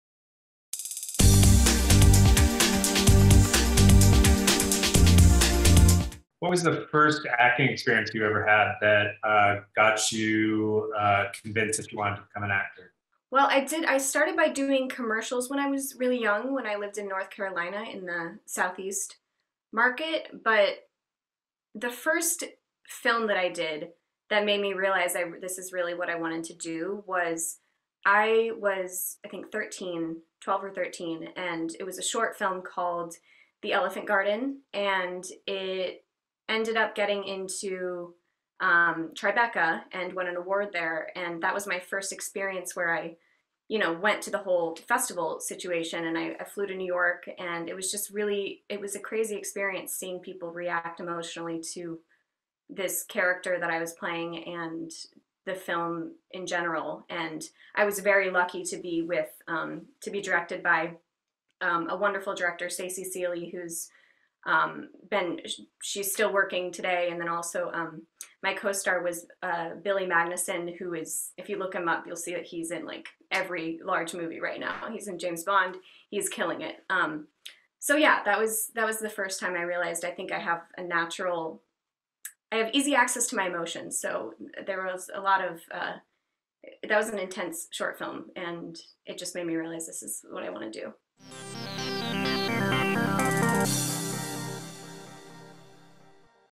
Kelley Mack Bio: How I Got Into Acting!
Kelley Mack Bio: How I Got Into Acting! Interview with Erik Meers | For more Kelley, go